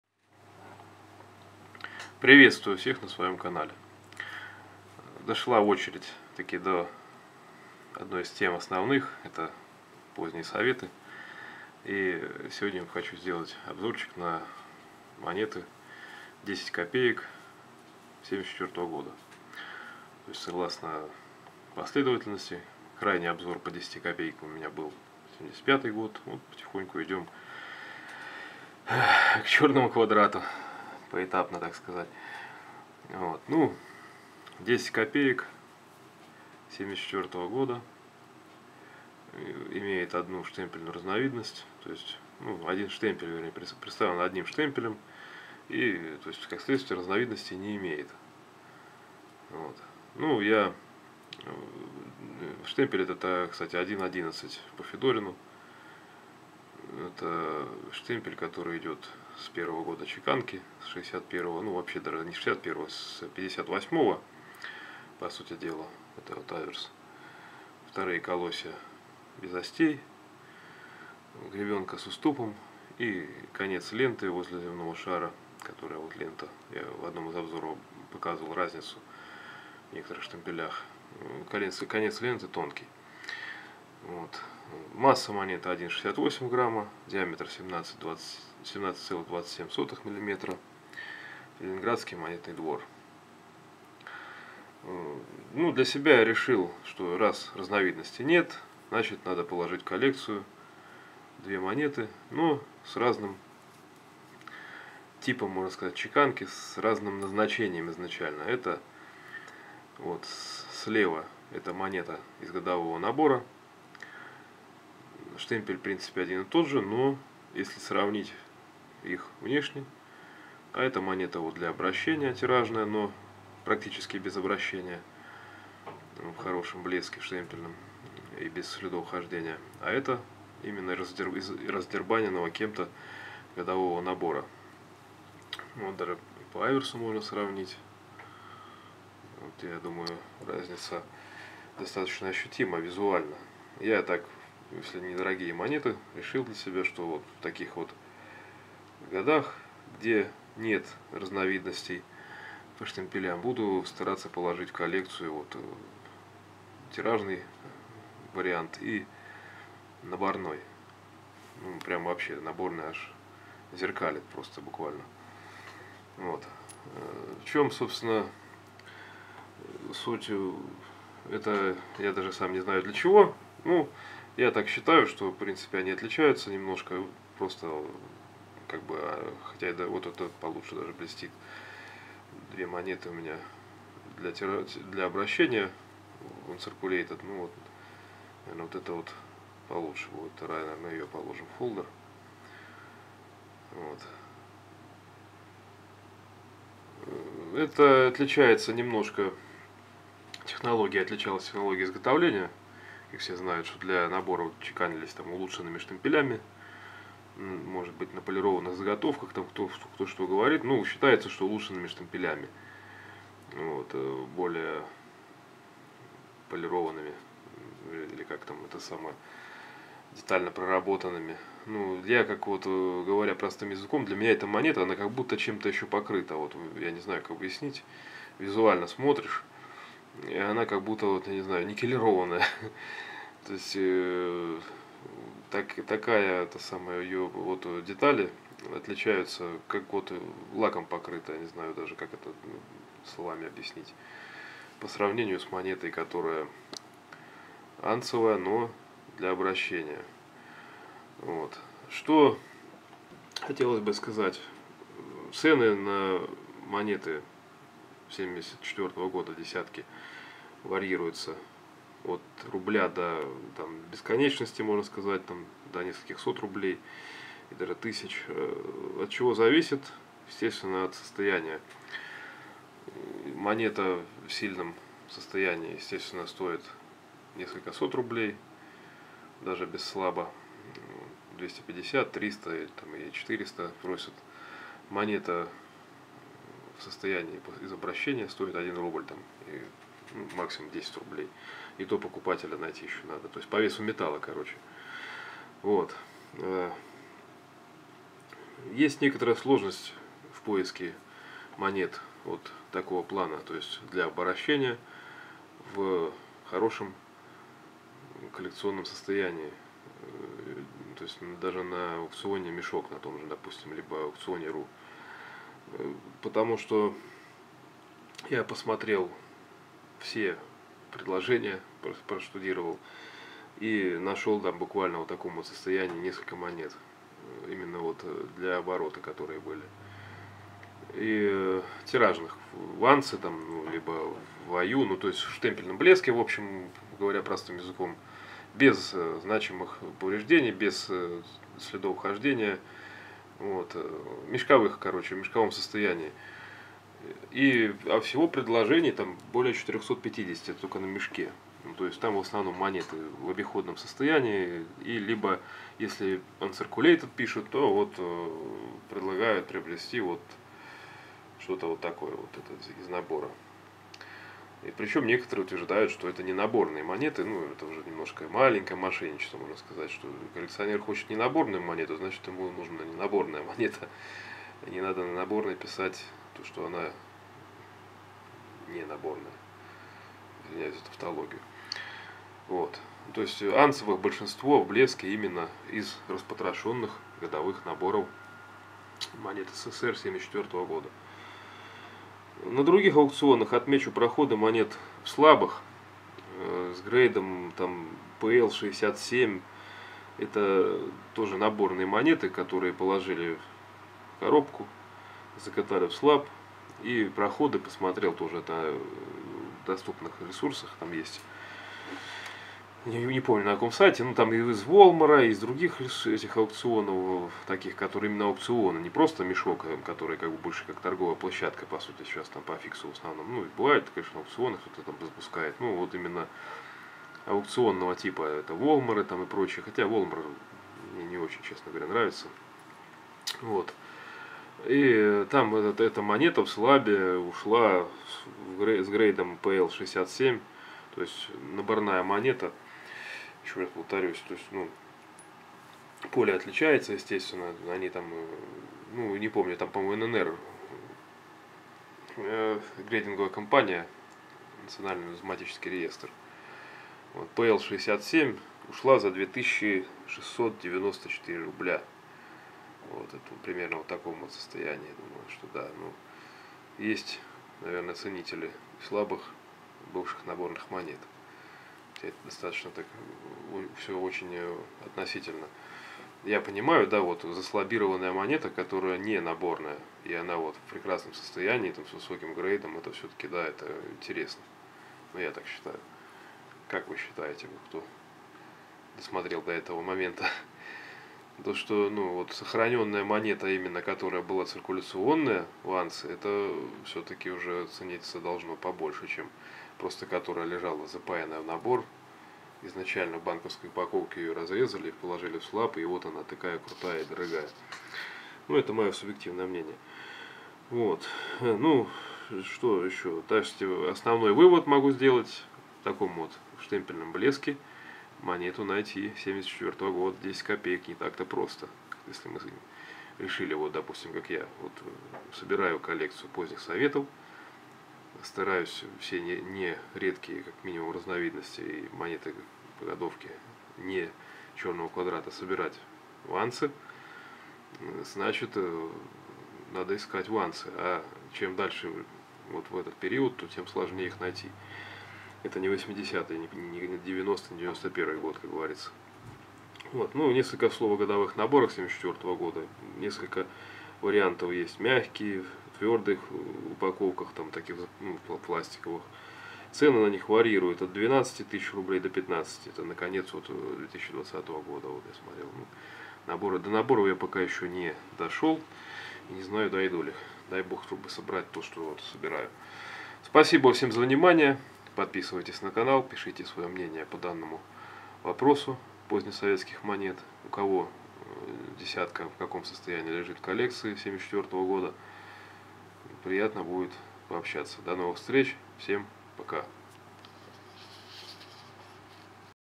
Монеты СССР. 10 копеек 1974 года-Обзор монеты и анализ цен
Просто ли найти сейчас гривенник 1974 года для своей коллекции в достойном состоянии? Сколько стоит классная монета?